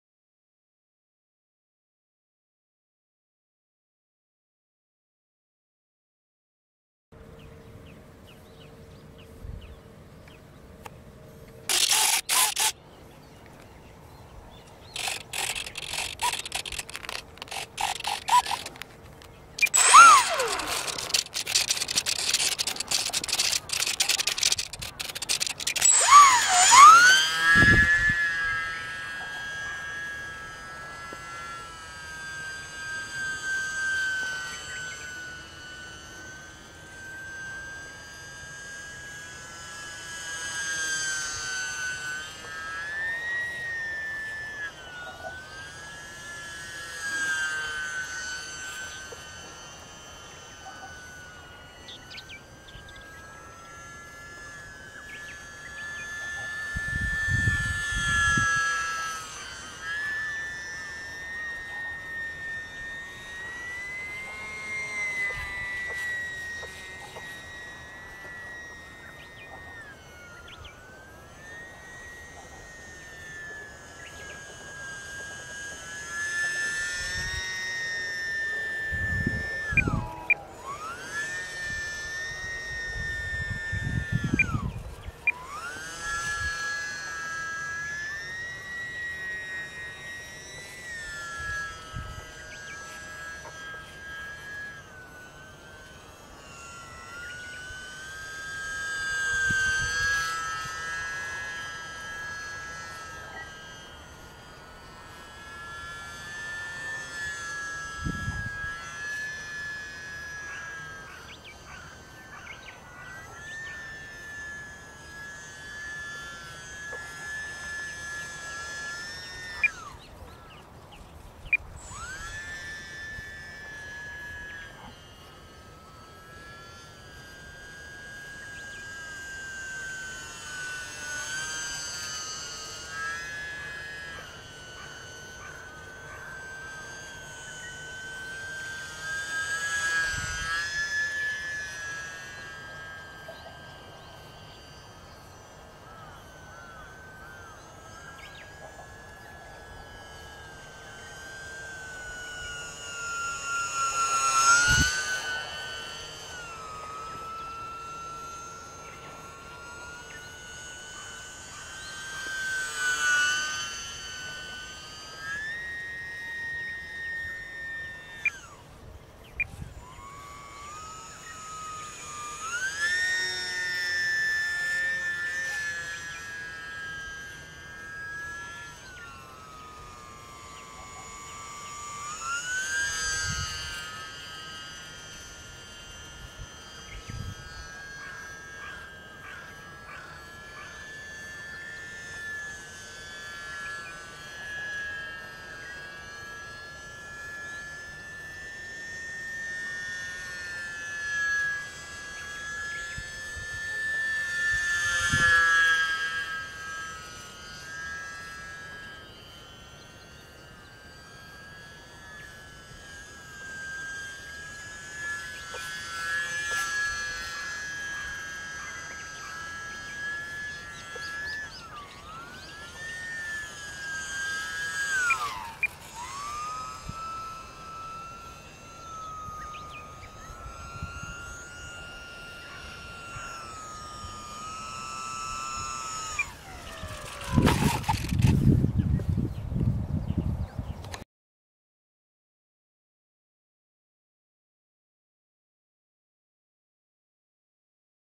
HORIZON HOBBY UMX F4F Wildcat BNF Basic
パワーは十分なのですが、芝生での滑走離陸がつらかったので車輪を外しました。　「目隠し」が付属しているので、違和感がありません。 飛んでいる姿も自然です。